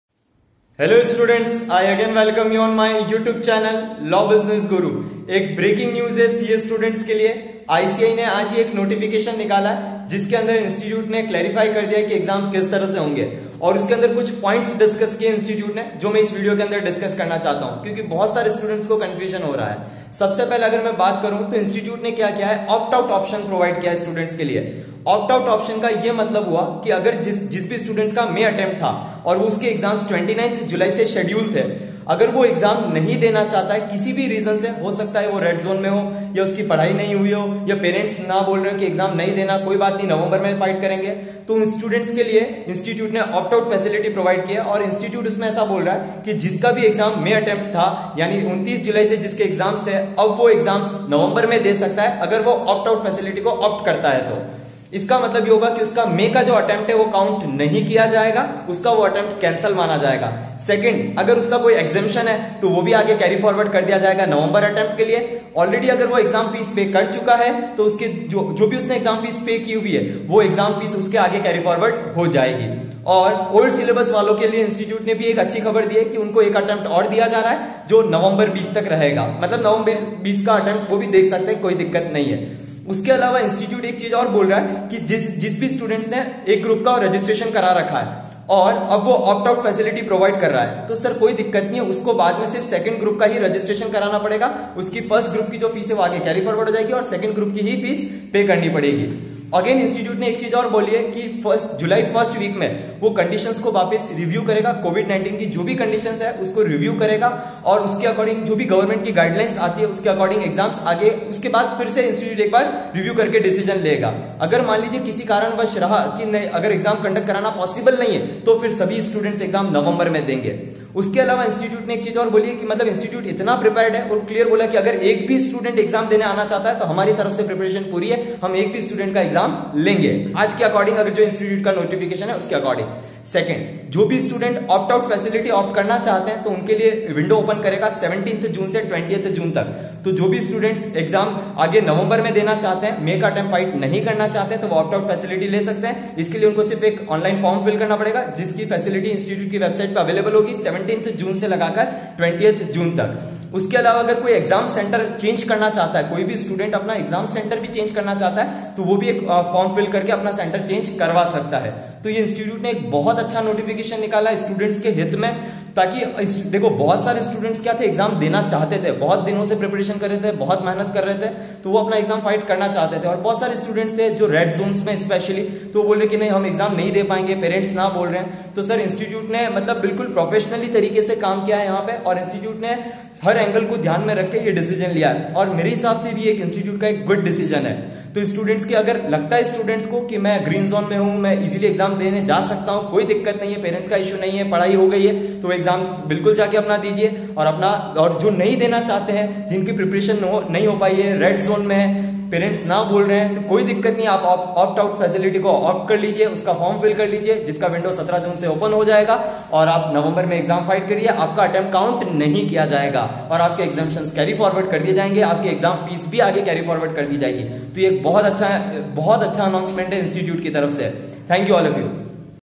V.V important announcement for CA students opt- out facility will be given to students
This video is all about opt - out facility will be given to students & attempt will not be counted